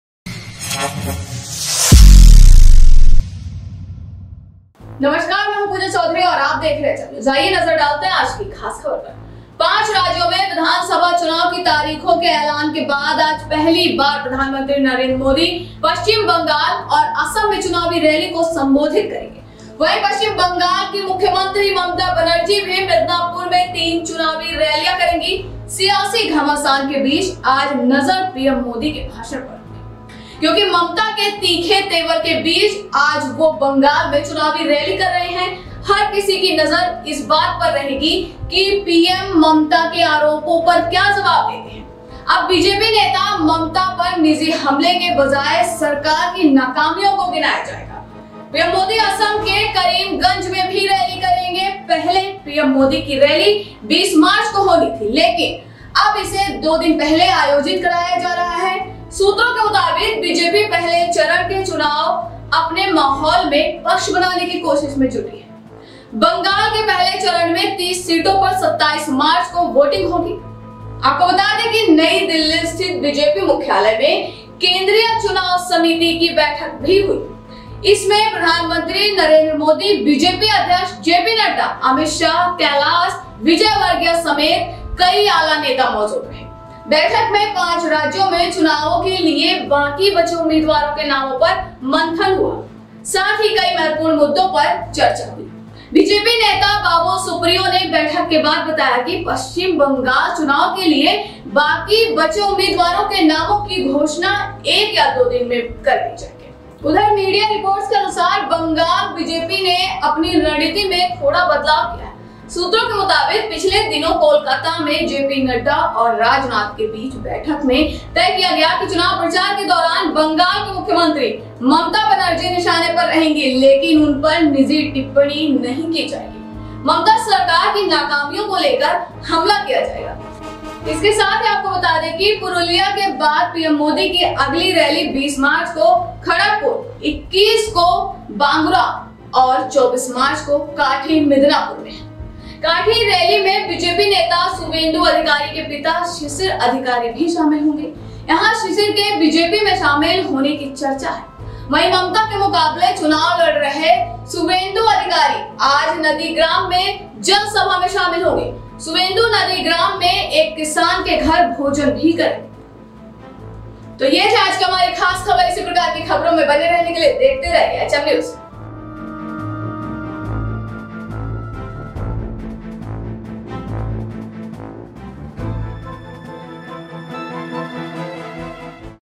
Bengal और Assam में PM मोदी की रैली | मिदनापुर में ममता भी करेंगी तीन चुनावी रैलियों को संबोधित
पांच राज्यों में विधानसभा चुनाव की तारीखों के एलान के बाद आज पहली बार प्रधानमंत्री नरेंद्र मोदी पश्चिम बंगाल और असम में चुनावी रैली को संबोधित करेंगे। वहीं पश्चिम बंगाल की मुख्यमंत्री ममता बनर्जी भी मिदनापुर में तीन चुनावी रैलियां करेंगी। सियासी घमासान के बीच आज नजर पीएम मोदी के भाषण पर होगी, क्योंकि ममता के तीखे तेवर के बीच आज वो बंगाल में चुनावी रैली कर रहे हैं। हर किसी की नजर इस बात पर होगी कि पीएम ममता के आरोपों पर क्या जवाब देते हैं,अब बीजेपी नेता ममता पर निजी हमले के बजाय सरकार की नाकामियों को गिनाया जाएगा। पीएम मोदी असम के करीमगंज में भी रैली करेंगे। पहले पीएम मोदी की रैली 20 मार्च को होनी थी लेकिन अब इसे दो दिन पहले आयोजित कराया जा रहा है। सूत्रों के मुताबिक, बीजेपी पहले चरण के चुनाव अपने माहौल में पक्ष बनाने की कोशिश में जुटी है। बंगाल के पहले चरण में 30 सीटों पर 27 मार्च को वोटिंग होगी। apko bta de ki नई दिल्‍ली स्थित बीजेपी मुख्‍यालय में केंद्रीय चुनाव समिति (सीईसी) की बैठक भी हुई। इसमें प्रधानमंत्री नरेंद्र मोदी, बीजेपी अध्यक्ष जेपी नड्डा, अमित शाह, कैलाश विजयवर्गीय समेत कई आला नेता मौजूद रहे। बैठक में पांच राज्‍यों में चुनावों के लिए बाकी बचे उम्‍मीदवारों के नामों पर मंथन हुआ। साथ ही कई महत्‍वपूर्ण मुद्दों पर चर्चा हुई। बीजेपी नेता बाबुल सुप्रियो ने बैठक के बाद बताया कि पश्चिम बंगाल चुनाव के लिए बाकी बचे उम्मीदवारों के नामों की घोषणा एक या दो दिन में कर दी जाएगी। उधर मीडिया रिपोर्ट्स के अनुसार, बंगाल बीजेपी ने अपनी रणनीति में थोड़ा बदलाव किया है
Today, for the first time since the announcement of assembly election dates in five states, Prime Minister Narendra Modi will address election rallies in West Bengal and Assam. Meanwhile, West Bengal Chief Minister Mamata Banerjee will also hold three election rallies in Midnapore. In the midst of political turmoil, the focus will be on PM Modi's speech today, as he is holding an election rally in Bengal today amidst Mamata's sharp temper. All eyes will be on how the PM responds to Mamata's allegations, now the government's failures will be counted instead of a personal attack on BJP leader Mamata. PM Modi will also hold a rally in Karimganj, Assam. Earlier, PM Modi's rally was scheduled to be held on March 20, but now it is being held two days earlier. According to sources, the BJP is trying to form a party in its environment in the first phase of elections. In the first phase of Bengal, voting for 30 seats will take place on March 27. apko bta de ki The meeting of the Central Election Committee (CEC) was also held at the BJP headquarters in New Delhi. It was attended by Prime Minister Narendra Modi, BJP President JP Nadda, Amit Shah, Kailash Vijayvargiya and many other top leaders. The names of the remaining candidates for the elections in five states were brainstormed in the meeting. At the same time many important issues were discussed. BJP leader Babul Supriyo said after the meeting that the names of the remaining candidates for the West Bengal elections would be announced in a day or two. On the other hand, according to media reports, the Bengal BJP has made a slight change in its strategy